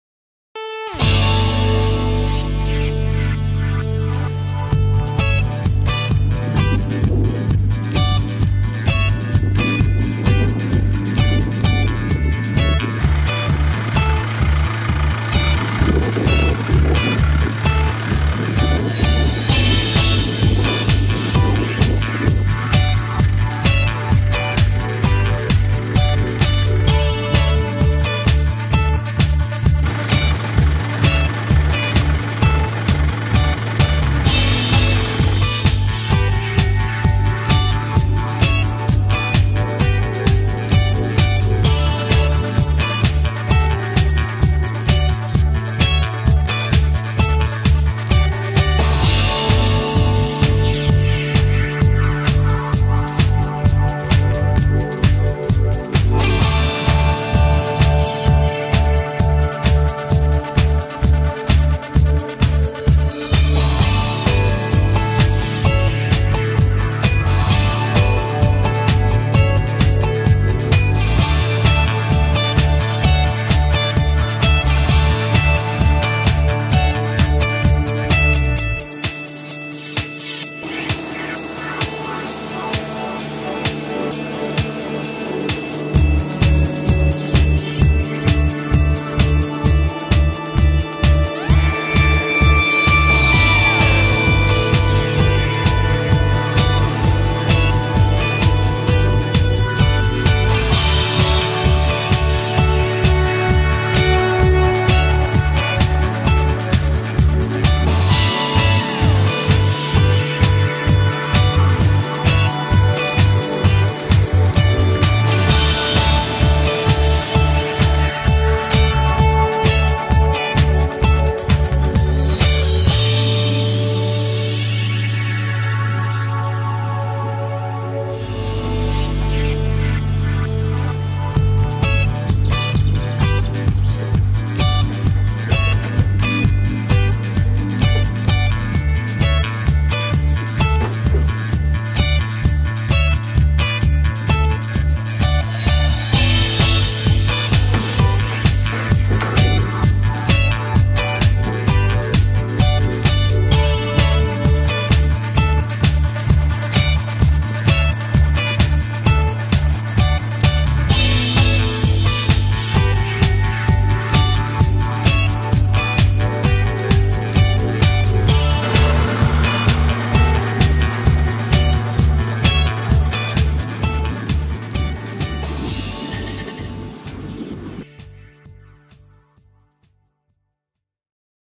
TRILO M4 Verticutting and collect soccer field
The M4 is a compact and economical vacuum sweeper with large radial tires for low soil pressure. You can choose between a 4 or 5-blade impeller. The impeller housing is fitted with a replaceable lining (wear plates) for extra long service life. The 5-blade impeller is used when increased suction power is required, such as for mowing and scarifying.